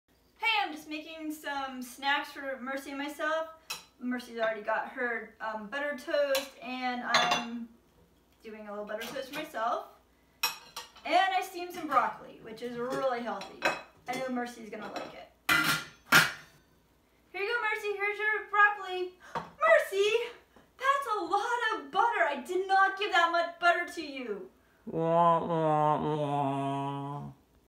Snack Time with Mercy (Watson)- A butter loving porcine wonder!
I just could not help myself in creating this silly video. Anyone who has read Mercy Watson books, knows the pig LOVES butter! I made this video with my current class in mind. They fell in love with the book series! I even converted the classroom dramatic play area into the Watson's kitchen. Students make toast, trick or treat with Mercy in her princess costume and write & deliver mail to the a classmail box (labelled 54 Deckawoo Dr). This year lots of art projects and literacy centers were created around the book series.